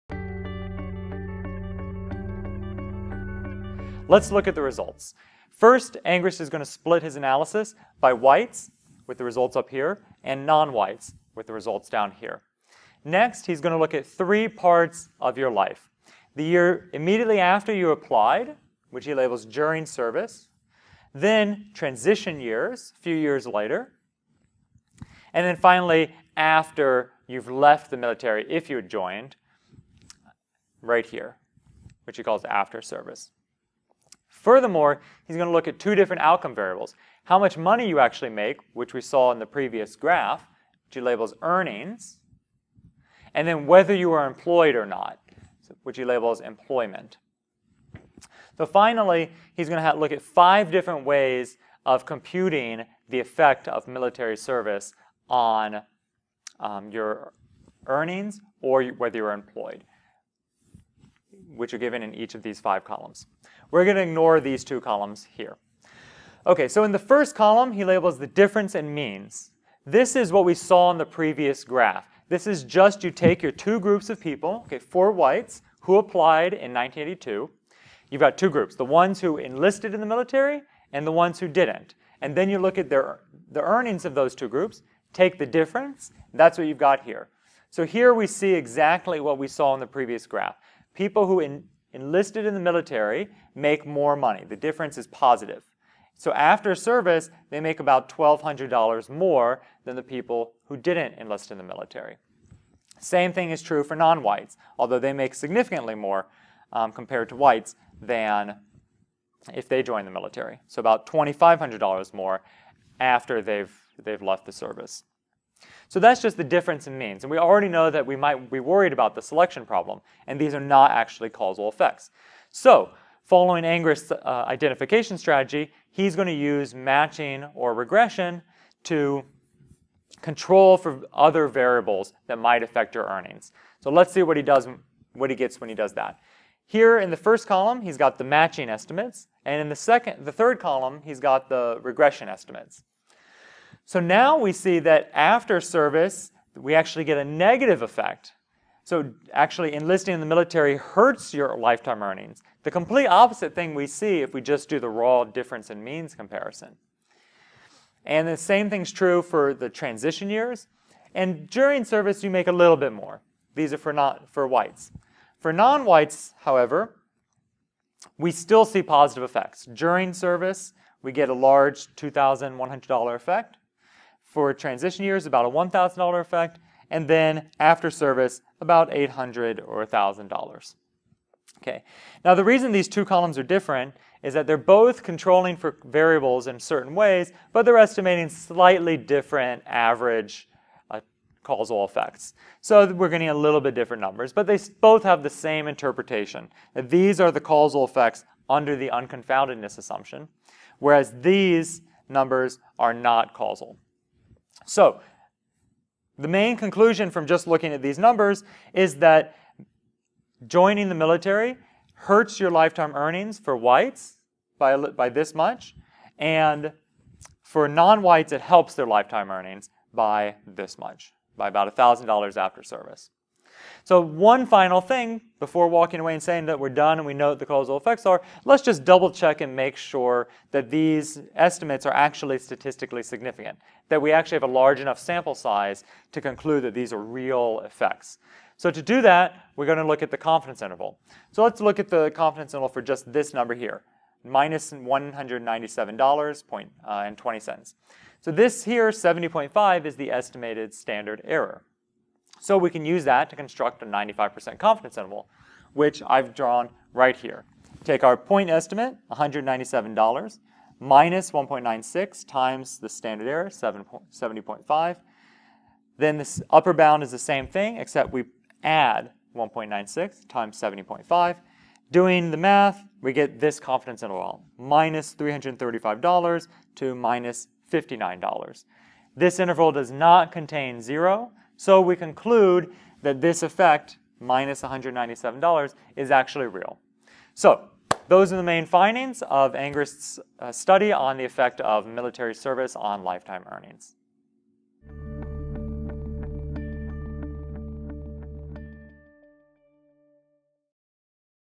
The Effect of Volunteer Military Service on Lifetime Earnings: Causal Inference Bootcamp
We conclude our discussion of Josh Angrist’s 1998 study by examining the causal effects computed via regression and matching.

Part of Duke University's Causal Inference Bootcamp: watch modules on this topic and much more